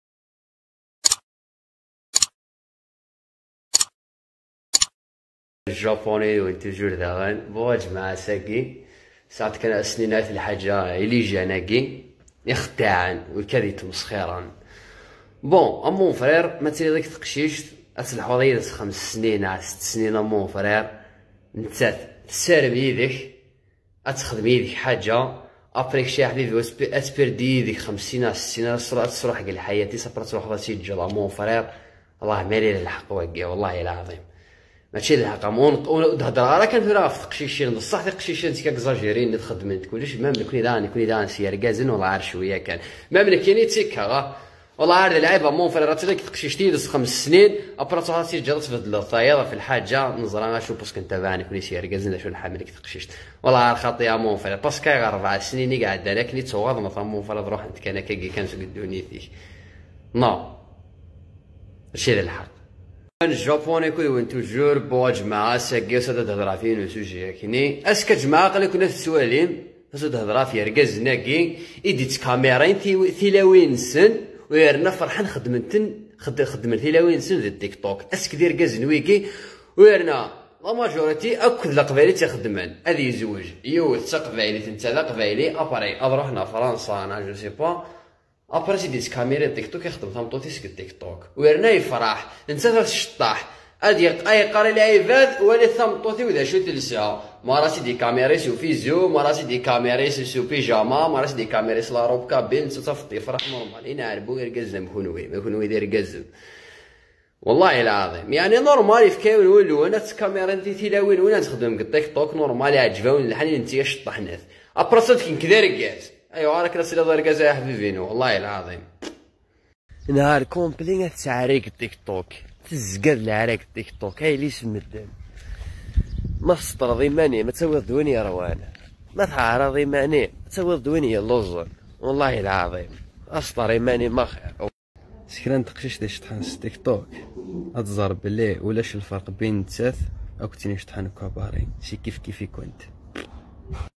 Le Japoni Kabyle Répond à Les Mecs ismenyiken ɣef Les Filles 💃😅
Un Jeune Kabyle Droit de Réponse
Un Kabyle Parle Sur Les Relations D'amour En Algérie
Un Kabyle Parle Sur Les Relations entre Les Kabyles